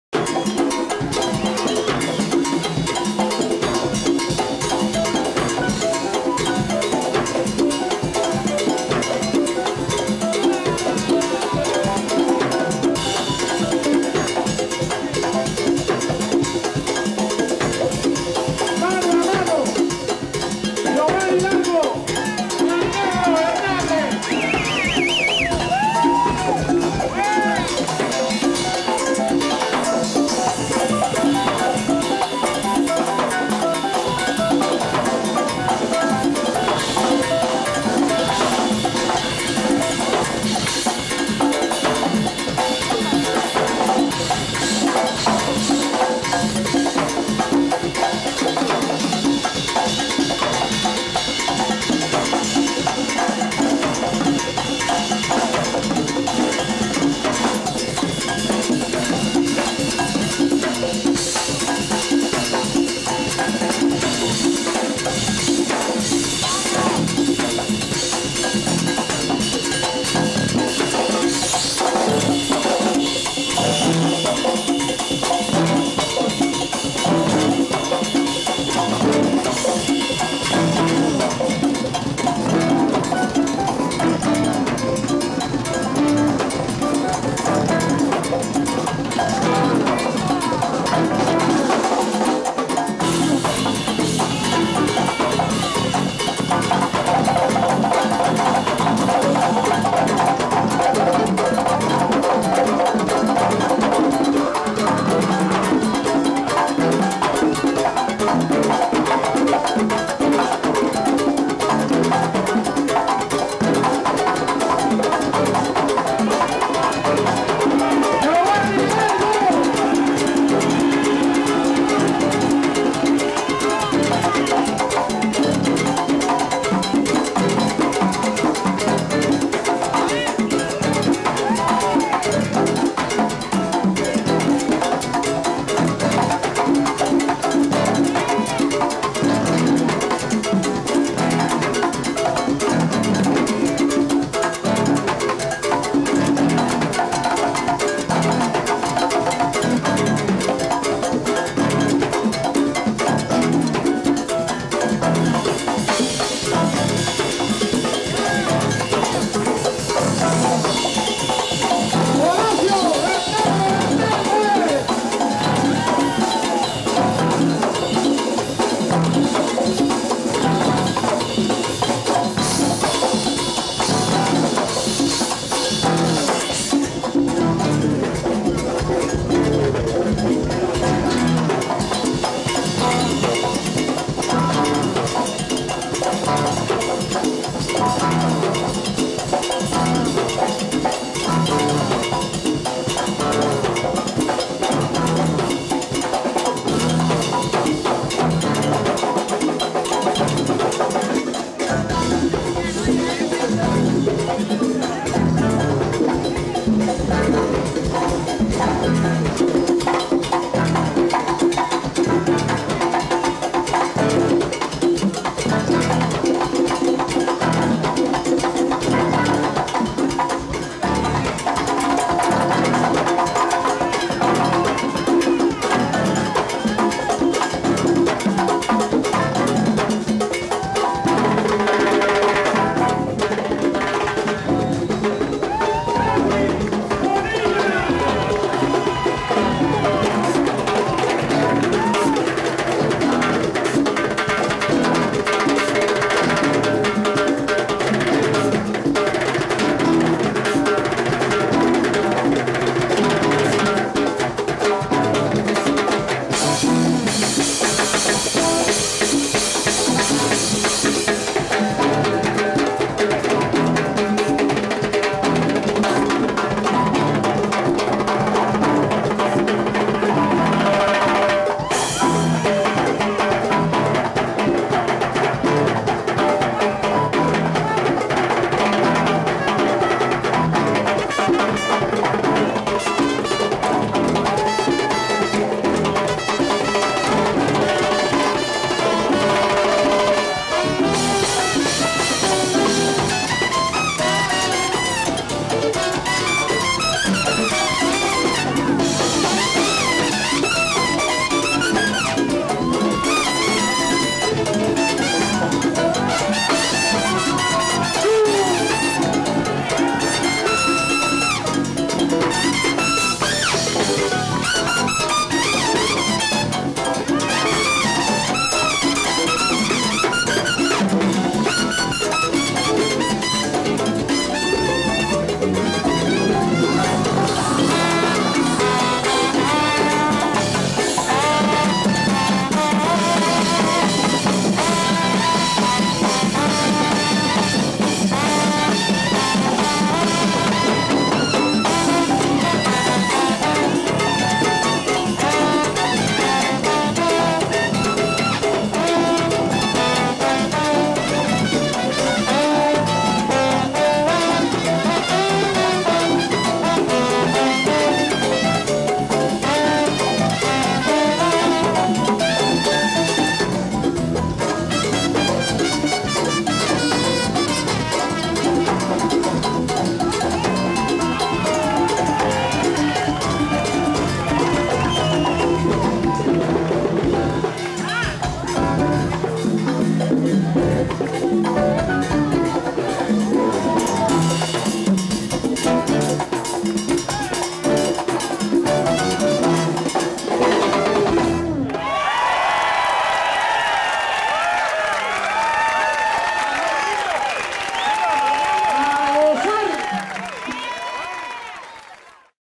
Miami Festival 2000 (3)
Recorded in Miami in 2000. Featuring Giovanni Hidalgo and friends playing some hot latin jazz number. I shot this with my Sony Hi 8 camera, I used Premiere Pro for editing. Part 3 of 3.